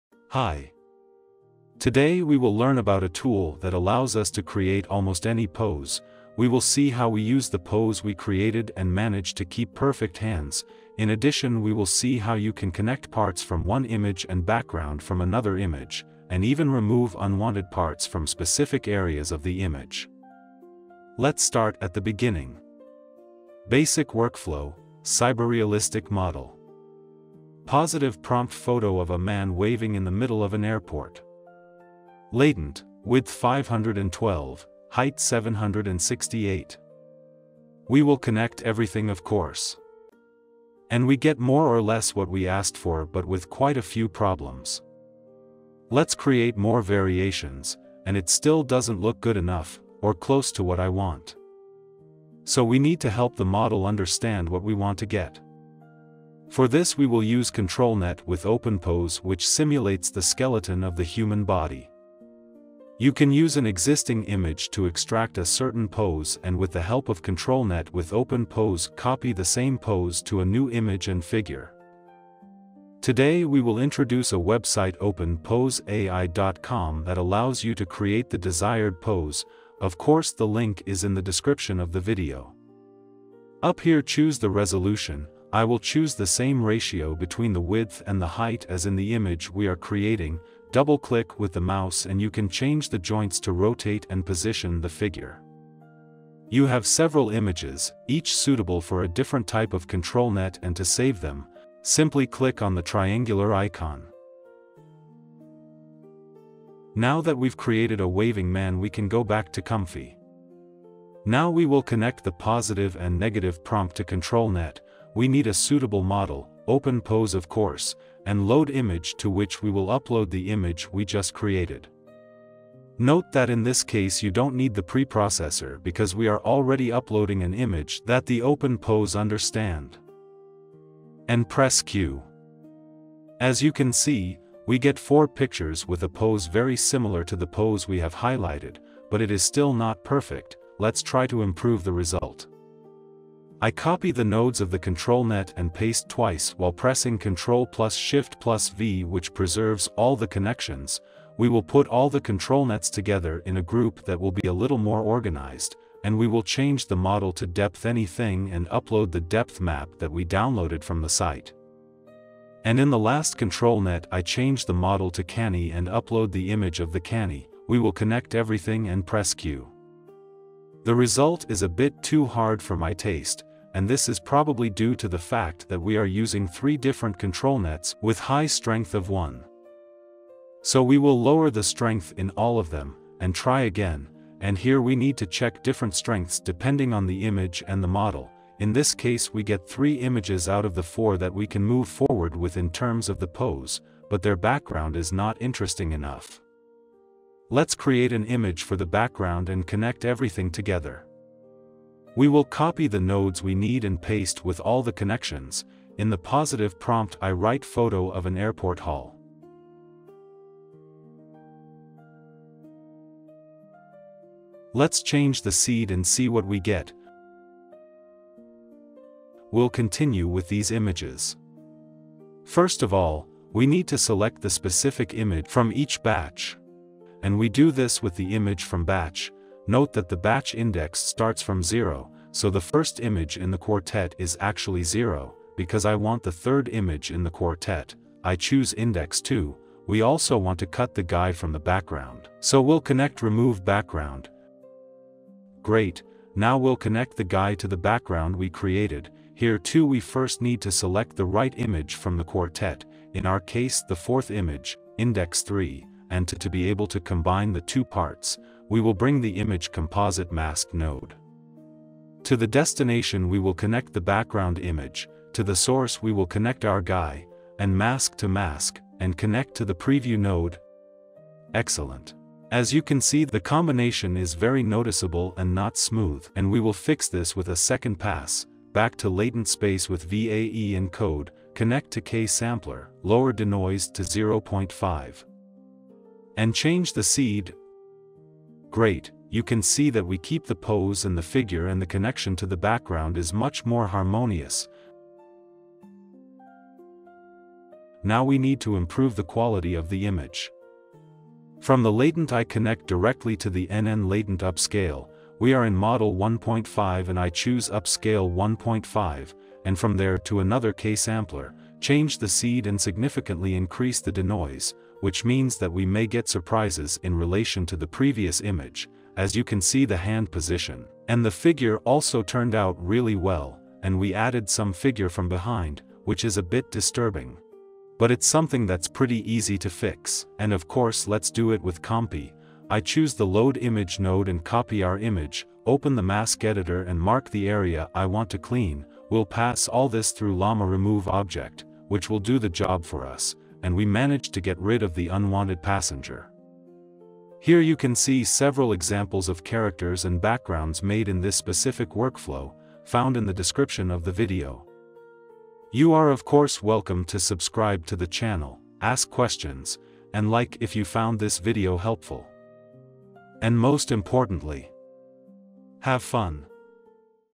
ComfyUI ControlNet OpenPose Composite workflow
Comfy-UI  ControlNet  OpenPose Composite workflow

In this video we will see how you can create any pose, and transfer it to different images with the help of ControllNets OpenPose DepthAnyThing and Canny
We will work with mask image composite node and composite a character from one image to different background, We will upscale the image using NNLatentUpscale node, And we'll see how you can remove certain objects from the image with the help of mask and LaMa Remove Object node